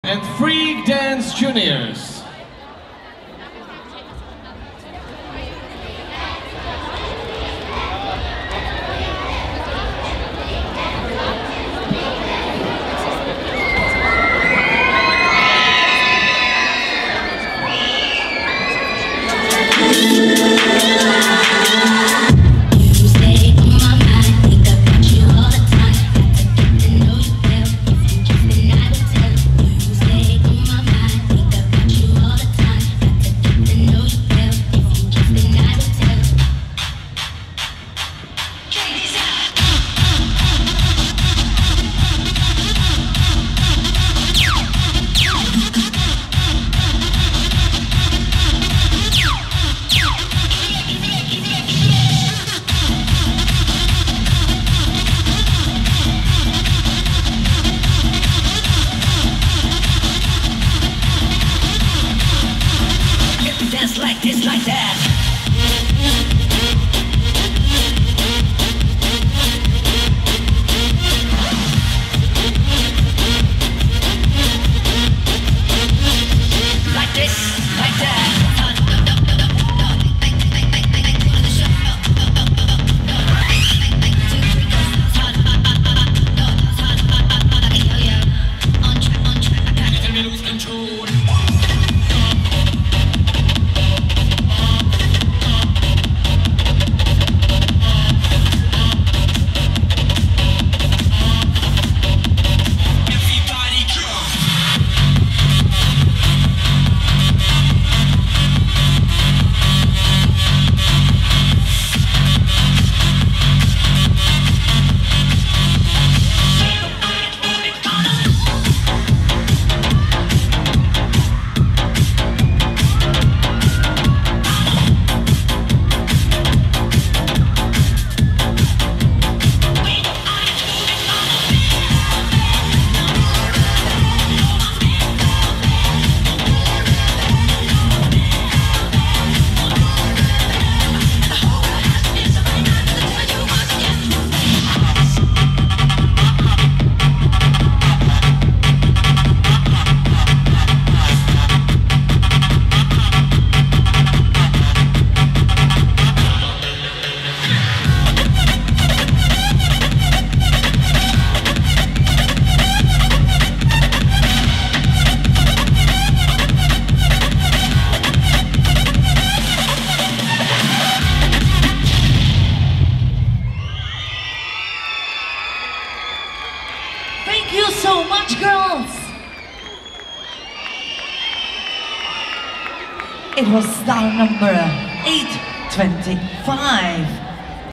CHOMUTOV 2022 ME IDO DD Formacje Juniorzy FREAK DANCE JUNIOR 11M
IDO EUROPEAN DISCO DANCE AND DISCO FREESTYLE CHAMPIONSHIPS
Chomutov  Czech Republic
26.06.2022

European Championship | Disco Dance | Formations | Juniors

FREAK DANCE JUNIOR - Dancetitle: DISCO DANCE
POLAND
11 miejsce